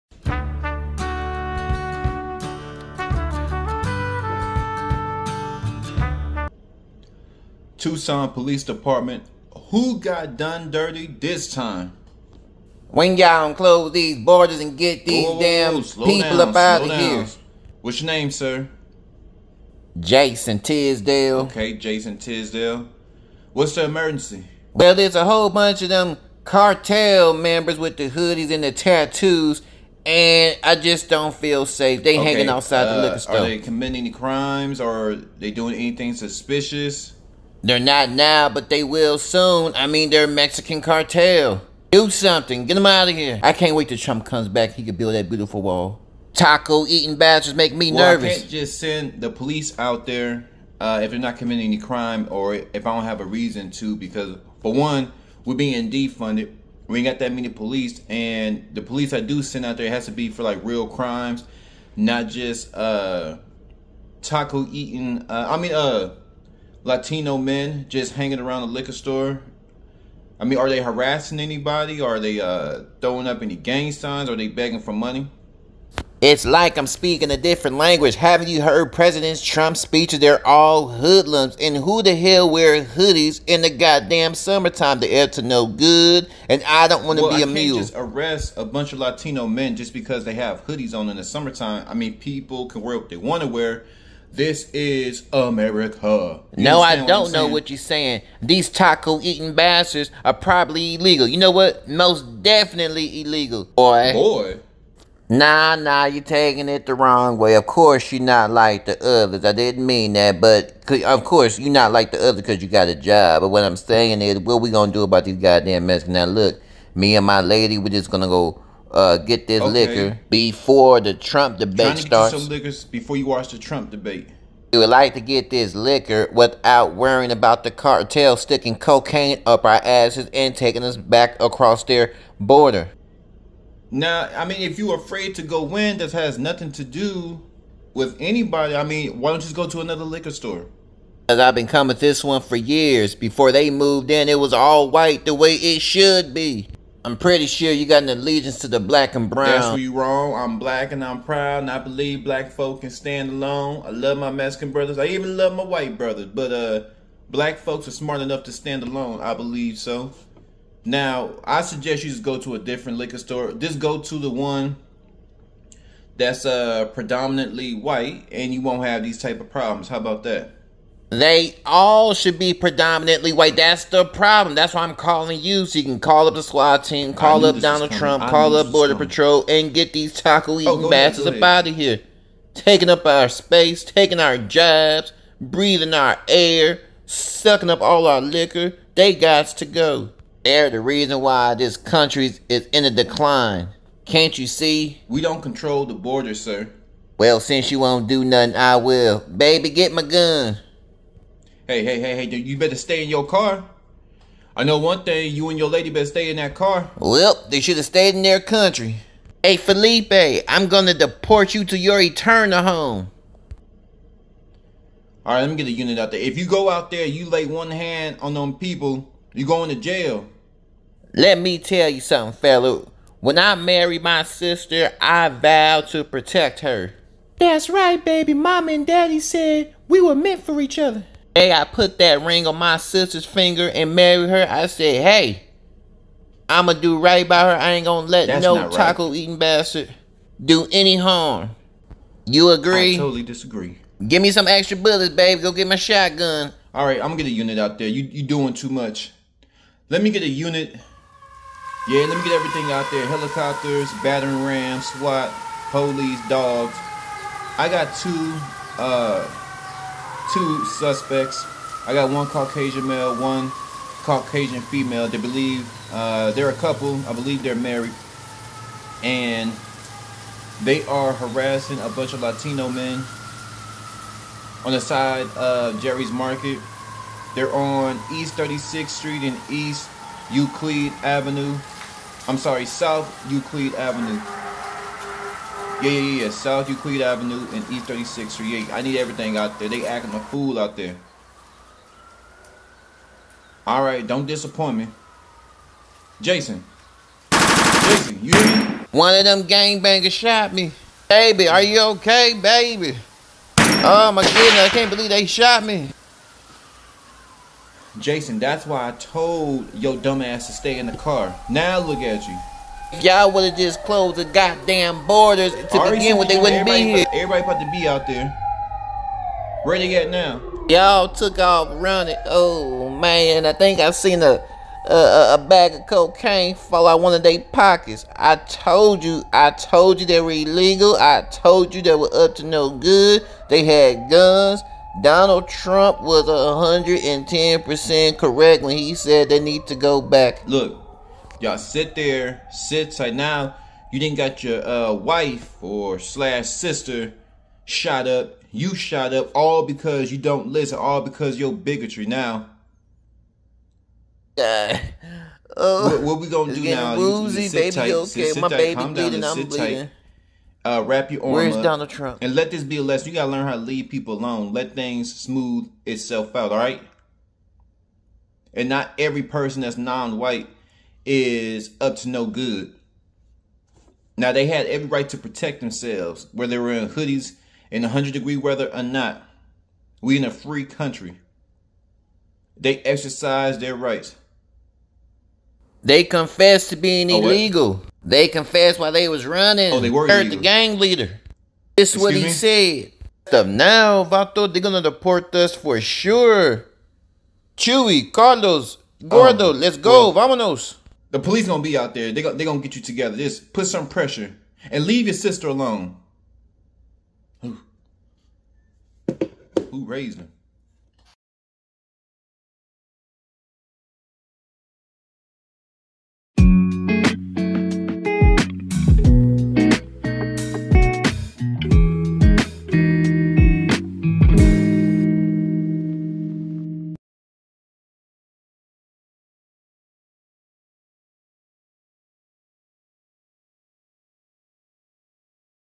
Trump Supporter Runs Up On A Group Of Latino Men Wearing Hoodies Goes Horribly Wrong. 🧐 comedy Ep.8
Trump supporter has lost his damn mind.